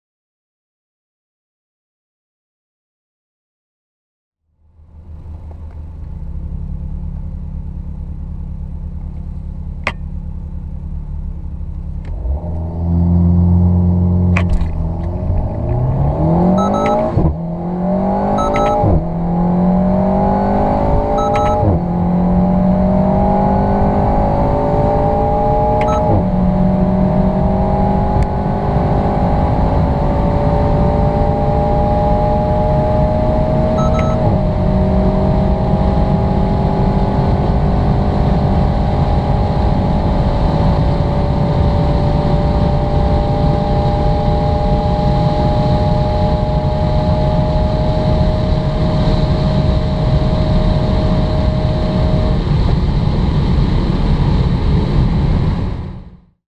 0-258 : new Alpine A110 acceleration top speed
Revivez notre test d'accélération sous régime du Launch Control à bord de l'Alpine A110 sur autoroute allemande non limitée. Puis retrouvez notre essai au Nürburgring en accès libre